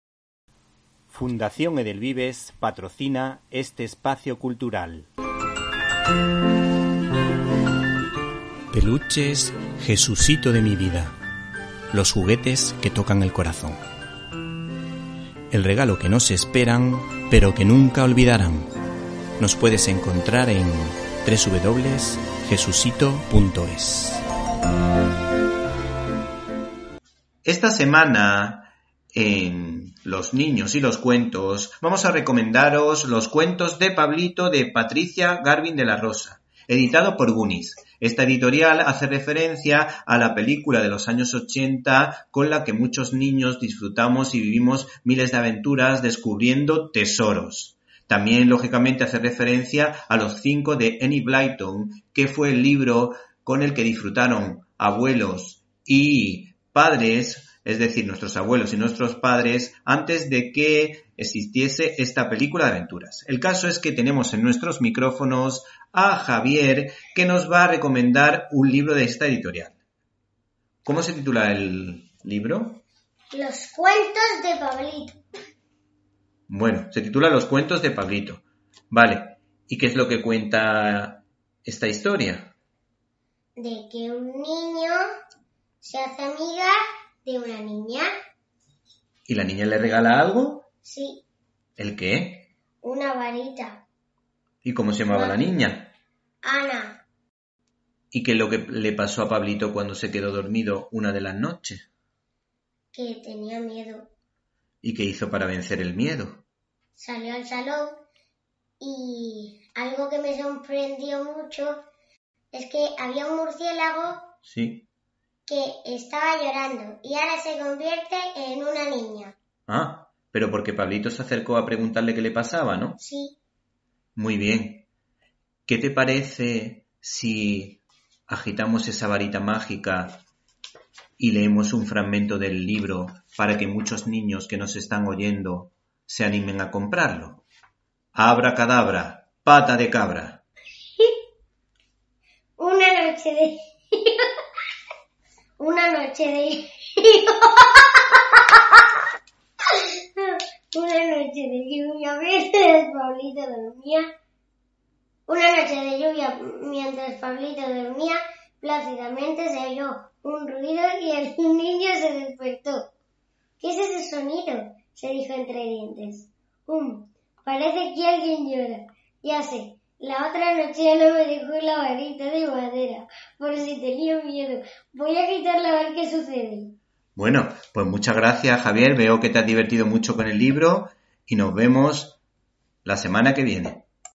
Los cuentos de Pablito-La magia de los niños en la radio
Los cuentos de Pablito-La magia de los niños en la radio. ¿Este es un libro que se puede encargar a los Reyes Magos?

Aquí puedes escuchar mi comentario...

La editorial Gunis nos habla de su libro:

Pablito, es un niño al que le suceden muchas cosas interesantes por la noche, mientras duerme. Conoce a muchos amigos y vive muchas aventuras junto a su gran amiga Anna y demás amigos.